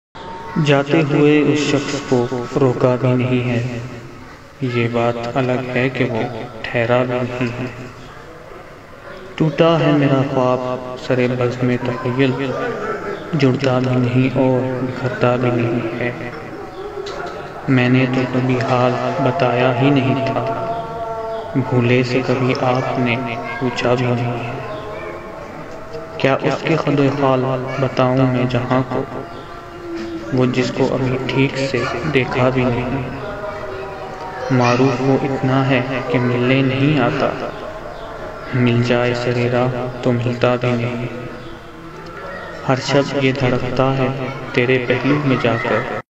Jate Hoy Us Shaks Ko.. poet [محمود احمد محور]
Jate Hoy Us Shaks Ko

  جاتے ہوئے اس شخص کو روکا بھی نہیں ہے
یہ بات الگ ہے کہ وہ ٹھہرا بھی نہیں ہے

ٹوٹا ہے میرا خواب سر بزم تخیل
جڑتا بھی نہیں اور بکھرتا بھی نہیں ہے

میں نے تو کبھی حال سنایا ہی نہیں تھا
بھولے سے کبھی آپ نے پوچھا بھی نہیں ہے

کیا اس کے خدو خال بتاؤں میں جہاں کو
وہ جس کو ابھی ٹھیک سے دیکھا بھی نہیں ہے

معروف وہ اتنا ہے کہ ملنے نہیں آتا
مل جائے سر راہ تو ملتا بھی نہیں ہے